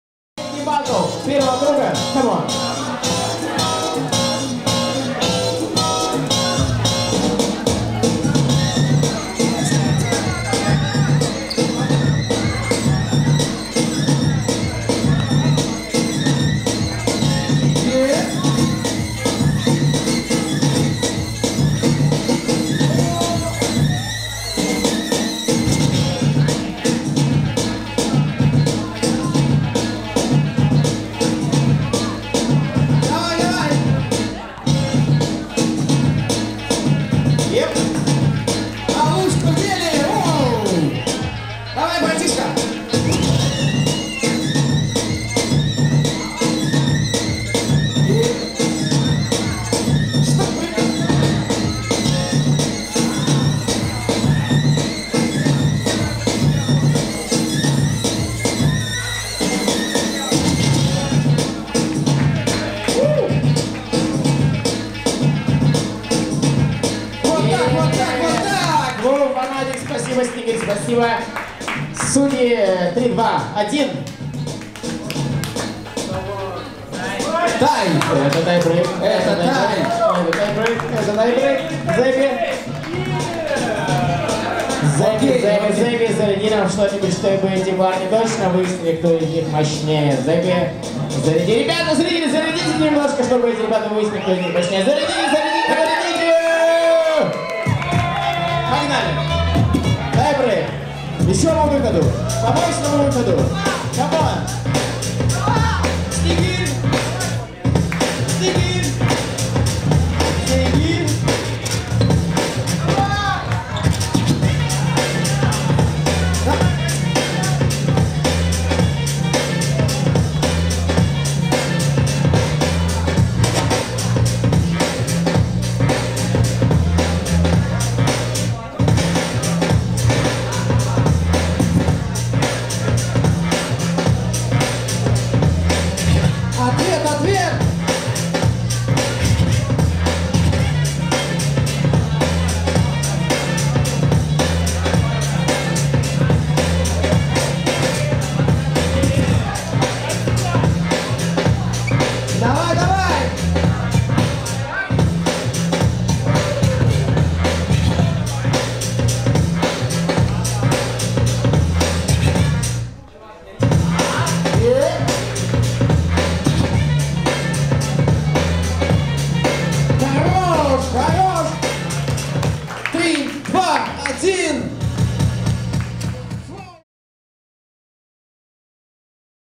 BBOY PRO BATTLE ↔ Round 2 ↔ 1'8 ↔ Dont Mind Flavour Jam 2020 dmfJAM bmvideo HD
Dont Mind Flavour Jam 2020 at the festival met bboy vs bboy and it was a hot battle! Nomination BBOY Judge DEMO is the nomination in which both dancers are not willing to give in! Each dancer has his own style of dance and battle! And only experience will show who truly deserves to be the first!